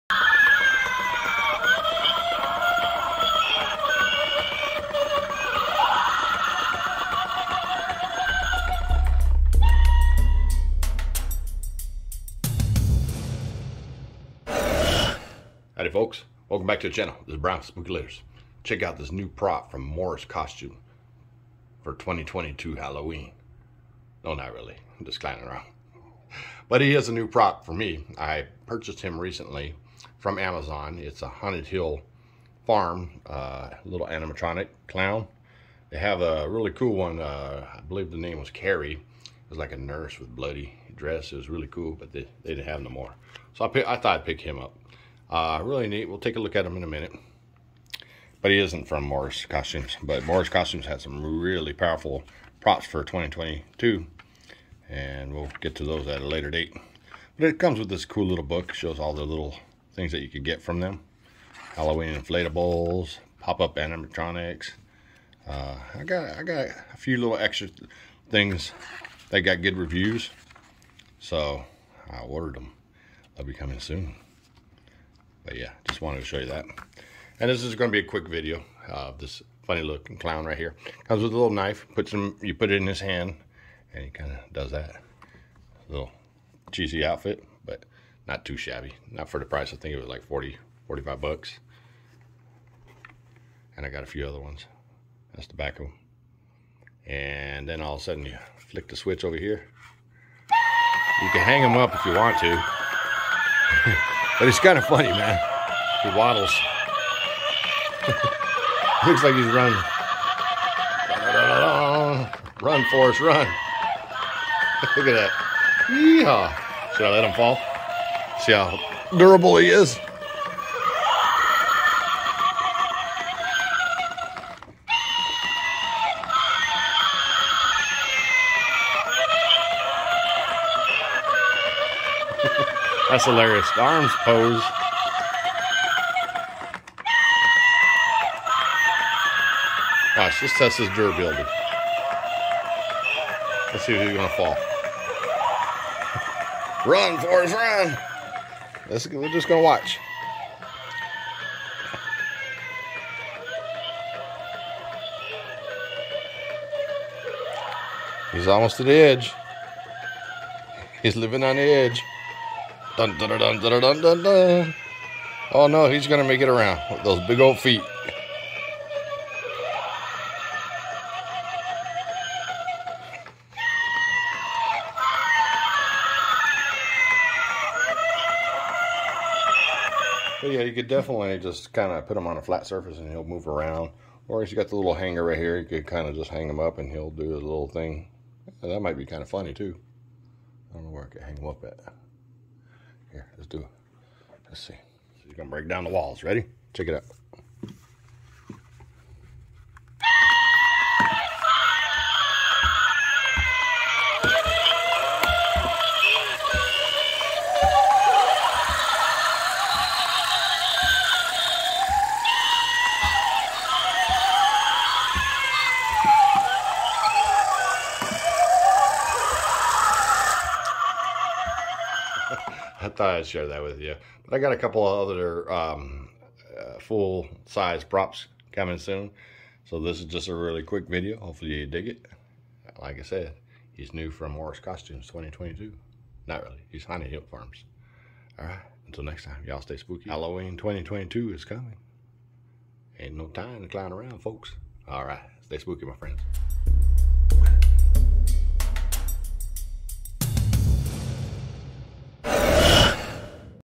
Scary Animatronic Clown Decoration for Halloween
Today we are taking a look at the Scary Animated Light-up Clown Decoration for Halloween from Haunted Hill Farm. This clown stands at a whopping 2.6 ft. He has Creepy Red LED Eyes | Touch Activated | Sound Effects and Poseable Arms | Battery-Operated. Well he's not really so scary but he is a super fun Scary Animated Lighted Clown Decoration for Halloween.

Halloween 2022 is coming and I hope to see some really cool prop ideas and animatronics!